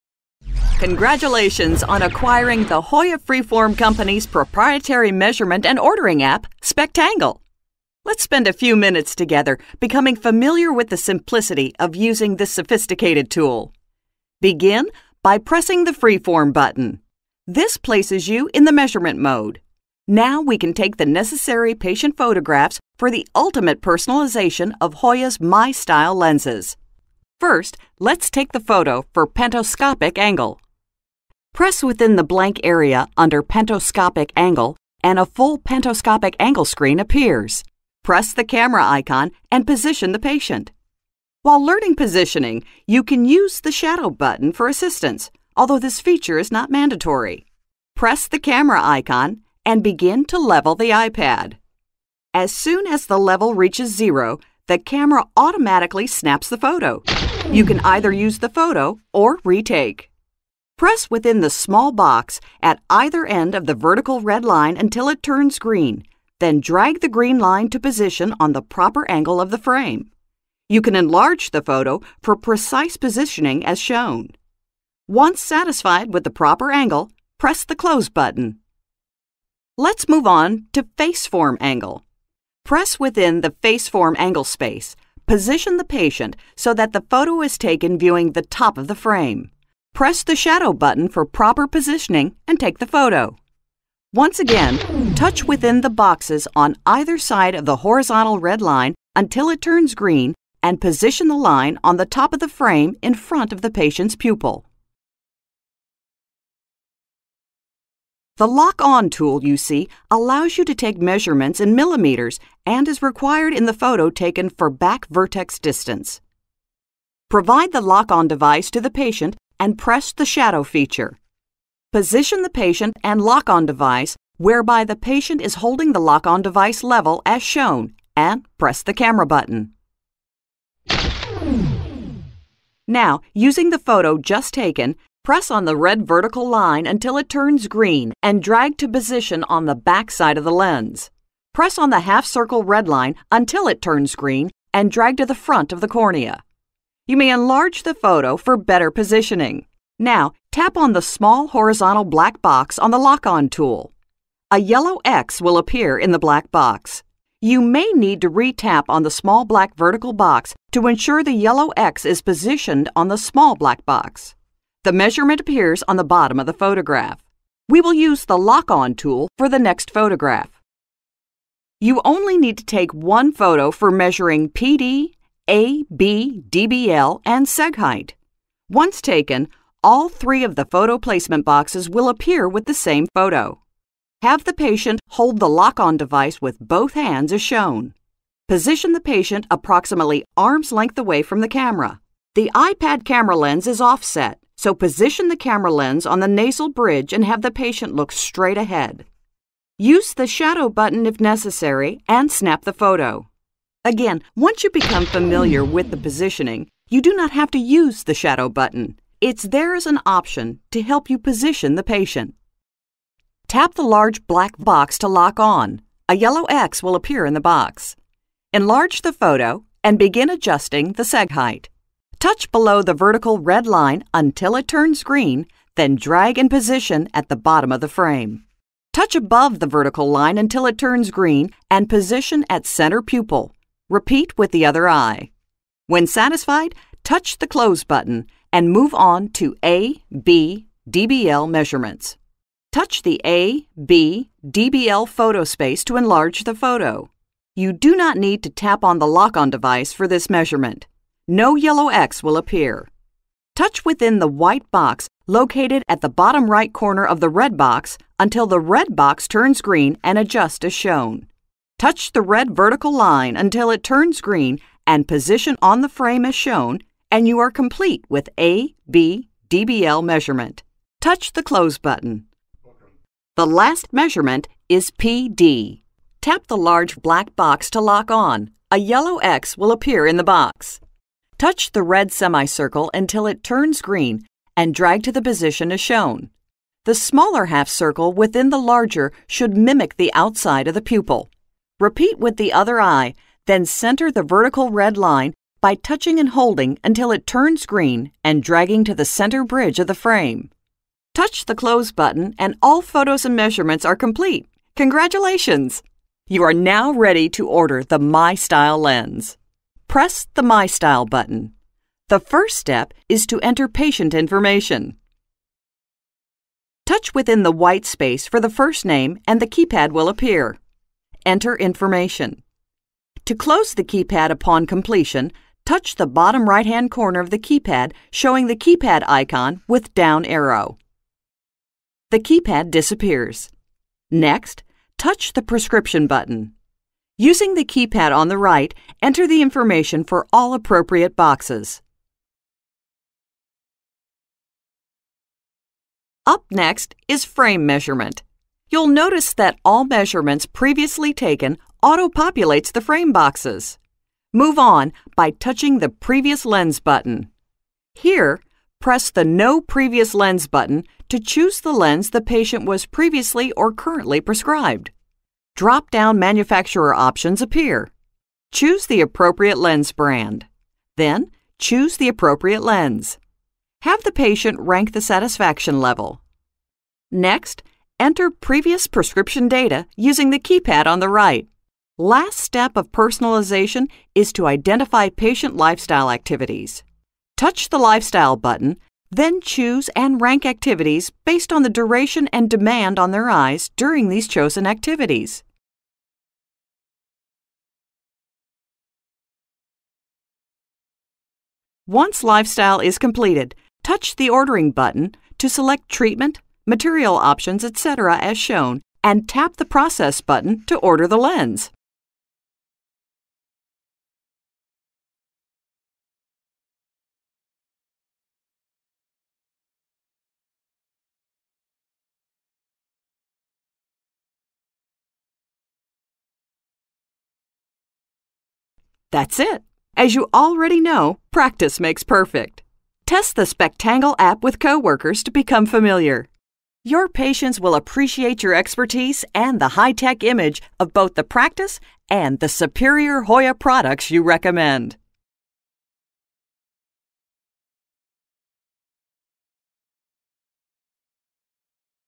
Full How To Use the HOYA Spectangle App Tutorial
The Spectangle app addresses many of the drawbacks required to prescribe personalized progressives including time necessary to take frame and fitting measurements, equipment investment and ease of operating measuring devices. Ideally, every dispensing table can now be equipped with an iPad3, the latest and greatest measurement technology available today.  Watch this instructional video to see how easy it is to use.